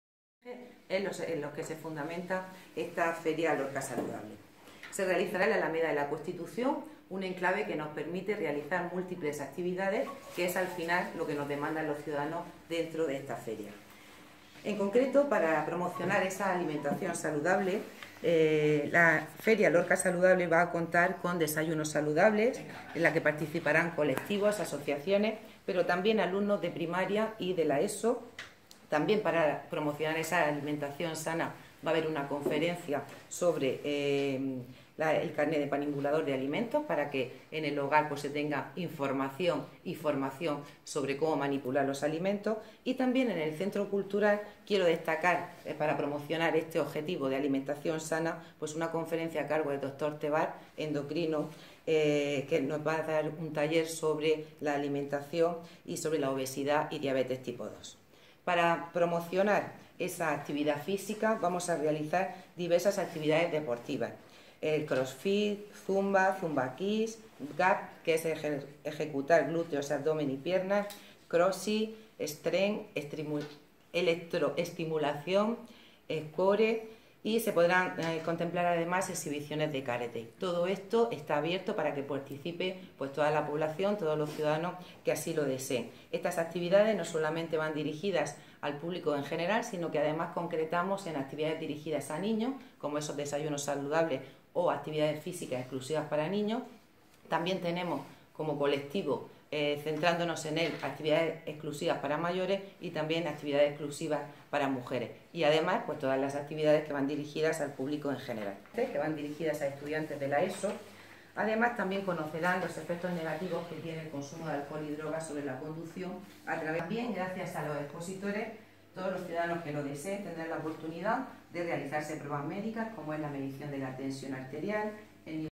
Lorca Saludable celebra su X aniversario estrena nueva ubicación en la Alameda de la Constitución
La feria se traslada a la Alameda de la Constitución dado el éxito de asistencia ciudadana que se venía incrementando año tras año. Los lorquinos podrán realizarse diferentes tipos de pruebas y disfrutar de talleres, charlas y actividades de animación deportiva.